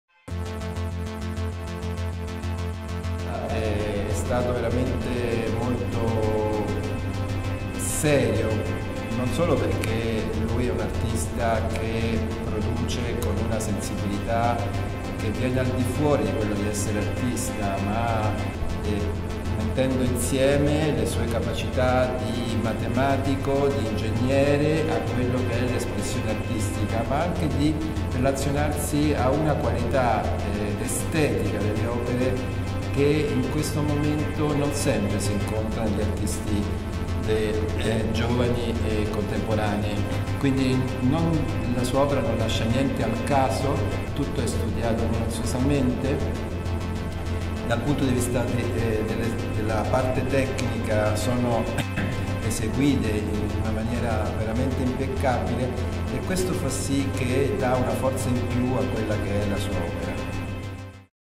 Moiréph - Rafael Parra Toro (PARRATORO) - Curated by Massimo Scaringella
A video showing some artworks by Rafael Parra Toro (PARRATORO) from "Moiréph" at Centro Cultural Borges. Voice and Curation by Massimo Scaringella.

Un video que muestra algunos trabajos de la muestra "Moiréph" en el Centro Cultural Borges (Buenos Aires - Argentina). La voz y curaduría de Massimo Scaringella.